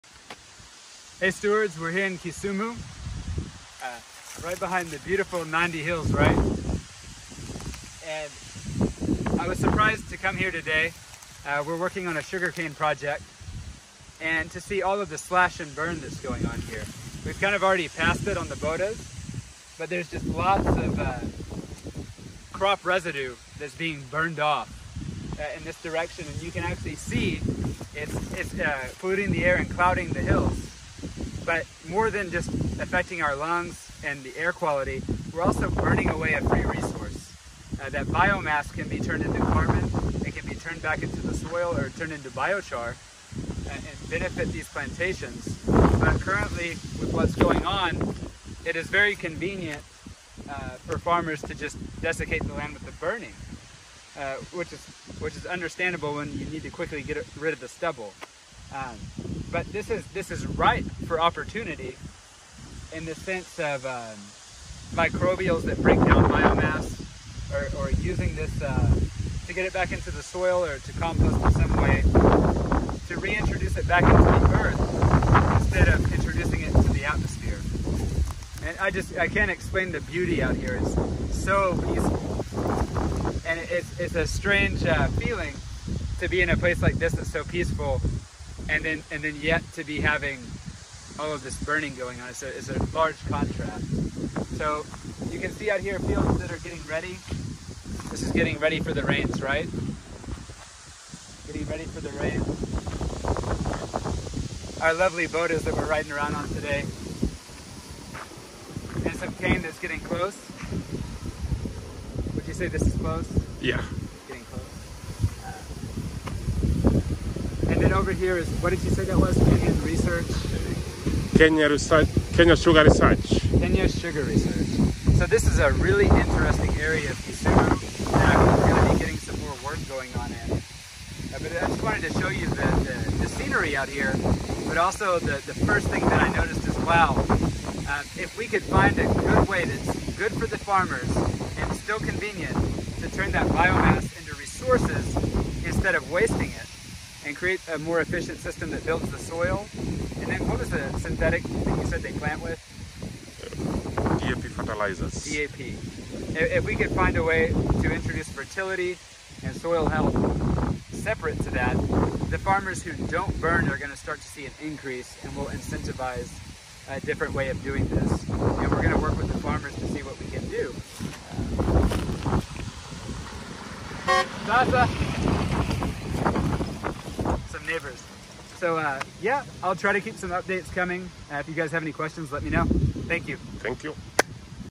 Organic Sugarcane Opportunities in Western Kenya
Spontaneous cell phone video update from the fields near Nandi hills outside Kisumu - lots of wind noise!  Phone was all we had , sorry but wanted to capture the surprise trip.  I wish I had stopped to get a good video of the burning off of the stubble , but maybe there will be a next time!  All of that haze is from burning fields not humidity.